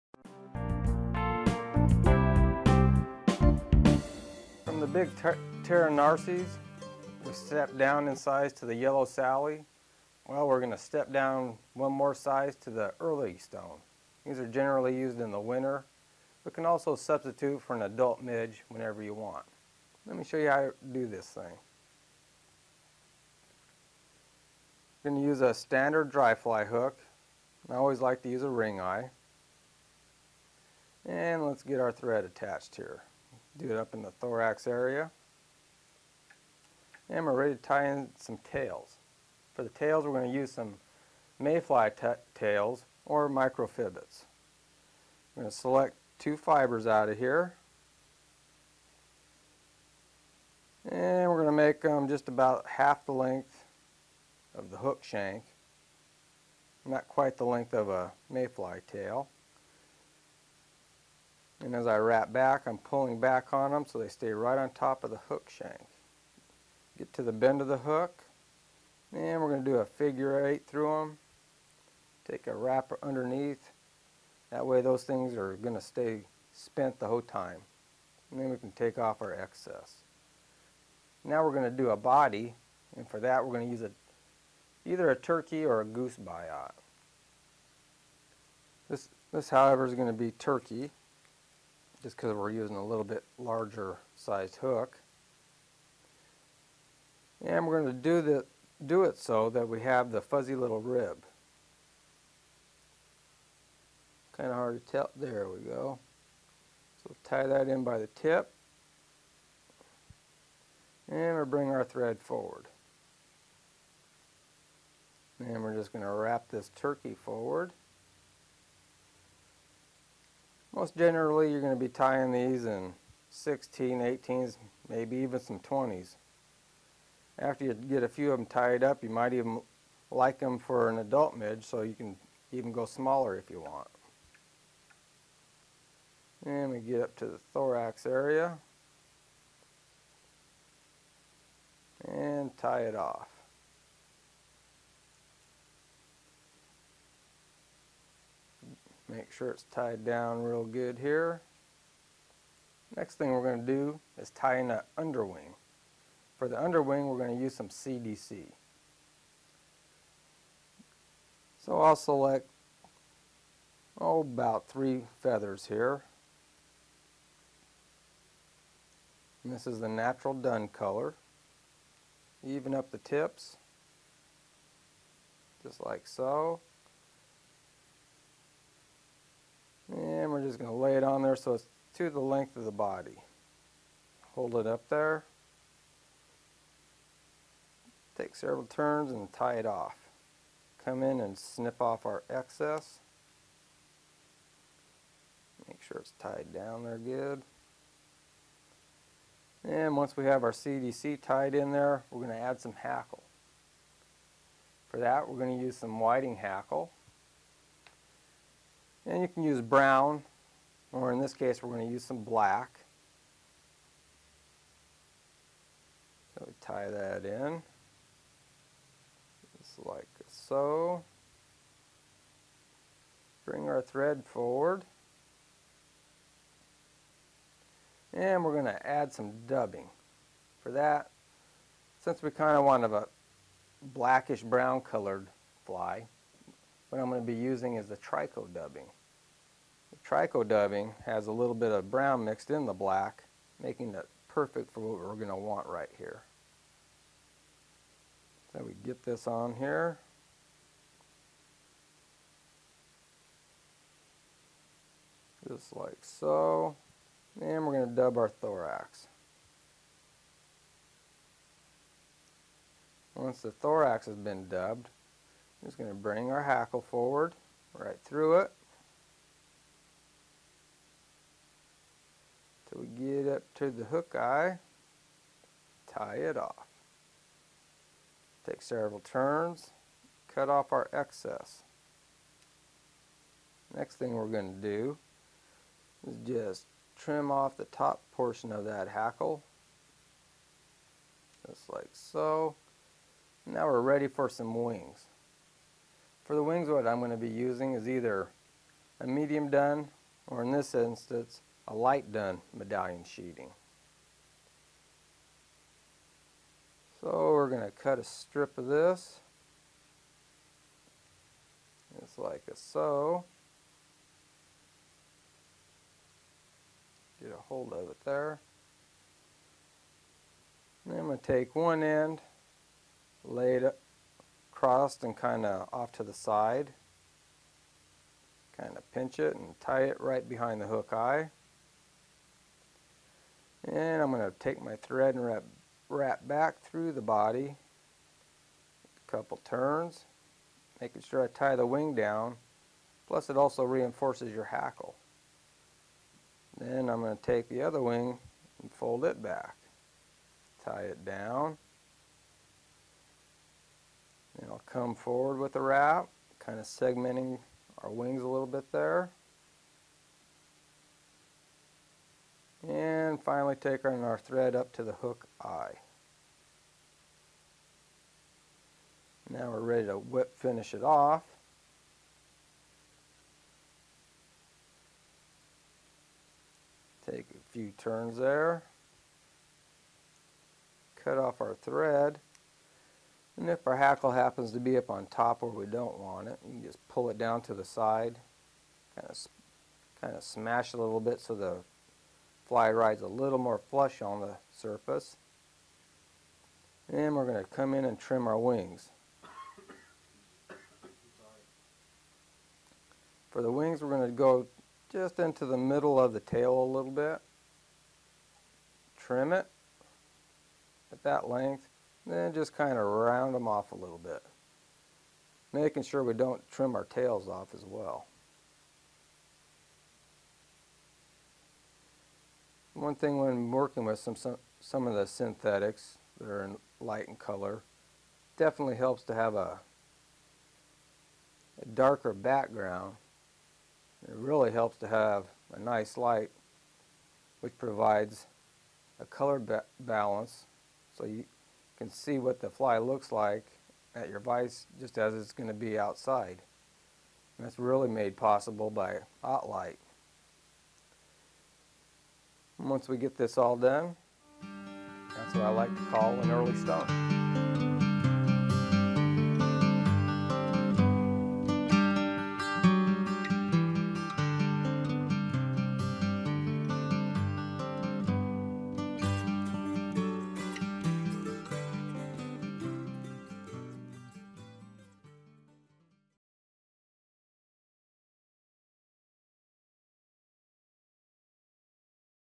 Early Dark Stone fly tying video instructions
The Early Stone is a good choice to use in the winter and as a substitute for an adult midge. Tied here by Shane Stalcup.